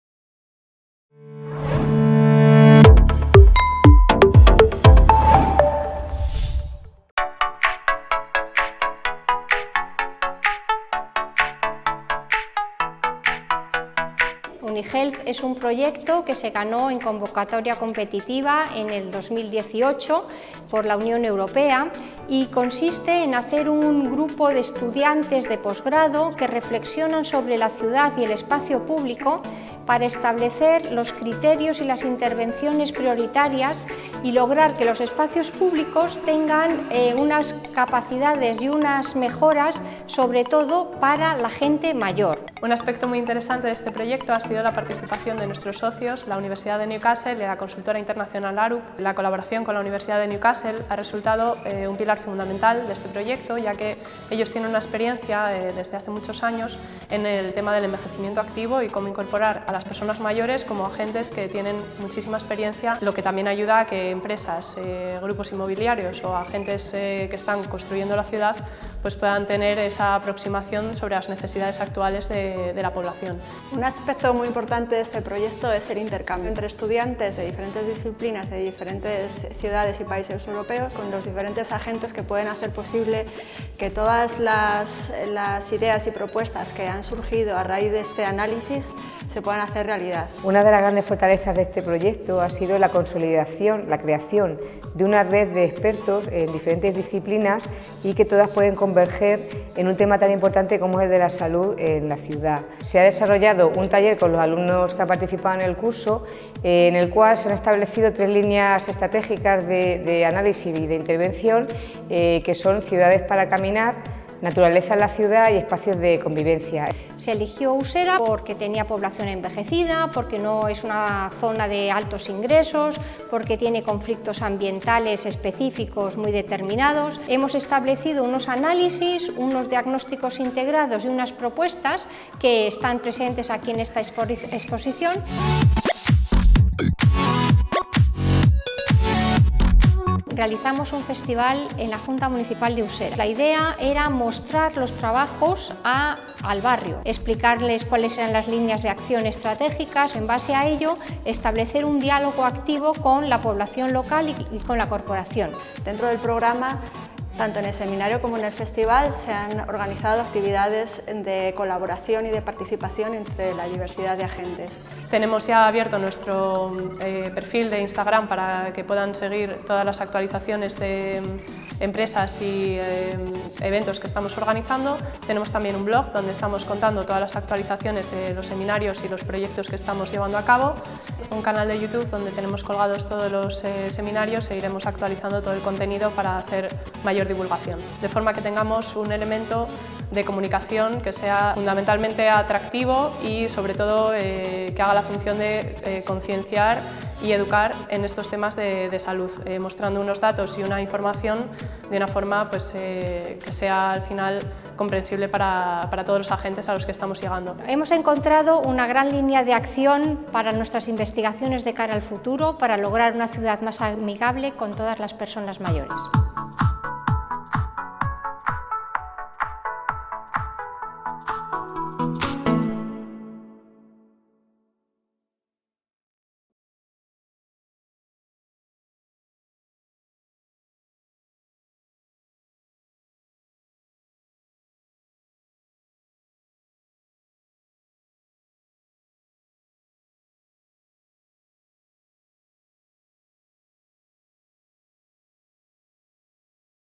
UNI-Health. Resumen
Se presentan las conferencias y las clases que configuraron el Curso de Formación Continua desarrollado en la ETS Arquitectura de Madrid, sobre Salud Urbana.